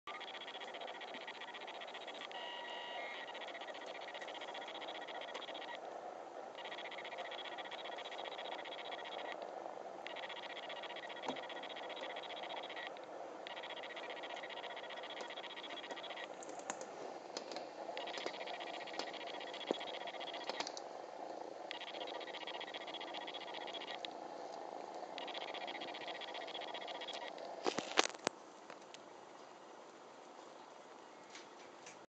オッパイ探しに･･･
足元から肩まで上がってきた子猫
腕を伸ばして渡らせました